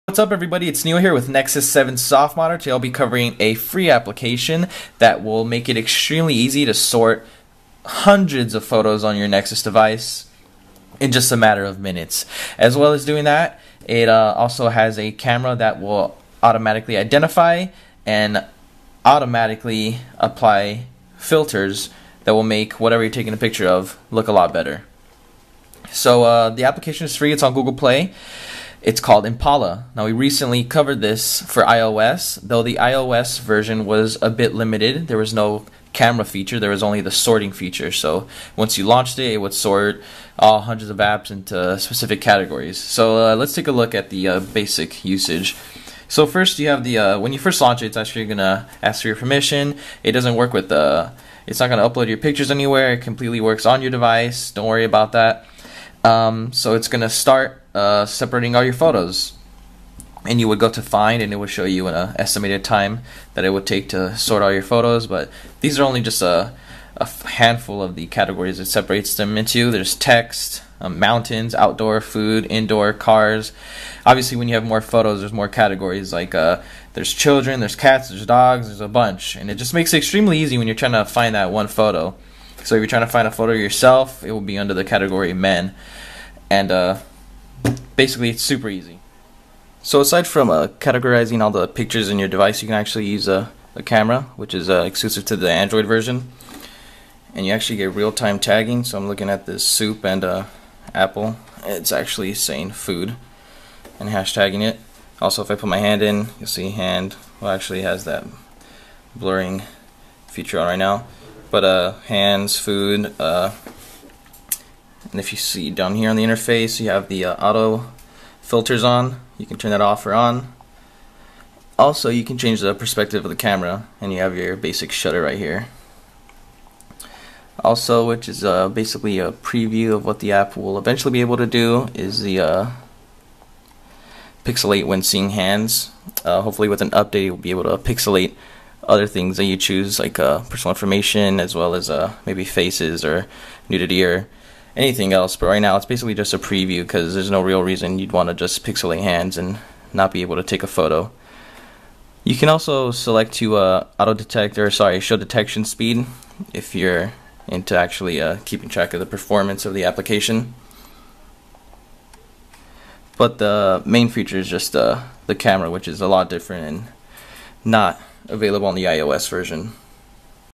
Auto-Identify & Sort All Your Photos on Your Nexus 7 Tablet [How-To]
How to Auto-Sort Old & New Pics [Nexus 7]

In this softModder tutorial, I'll be showing you an app called Impala that will sort every photo you have on your device into specific categories, in order to easily locate, as well as auto-filter and identify images when taking a photo.